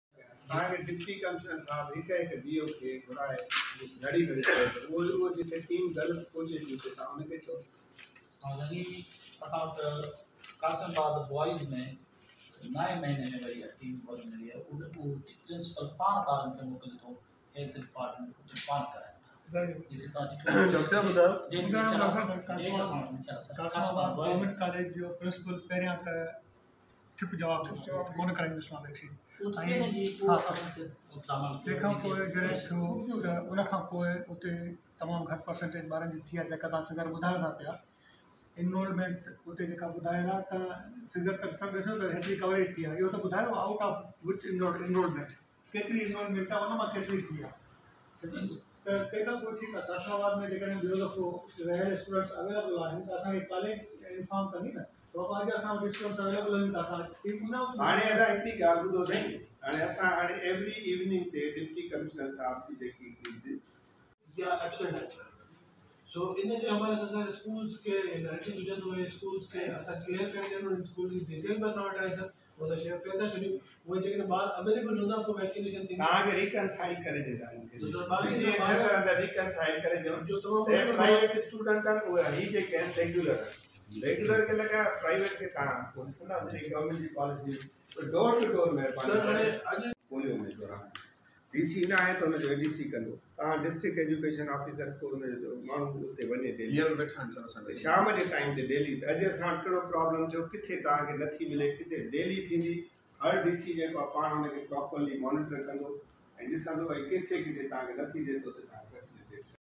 Covid Vaccine Drive In Hyderabad Sindh Slows Down.
Divisional Commissioner Hyderabad Muhammad Abbas Baloch chairs meeting with Deputy Commissioners and other officers and resented on slowing down of Covid Vaccine Drive in all the districts of the division.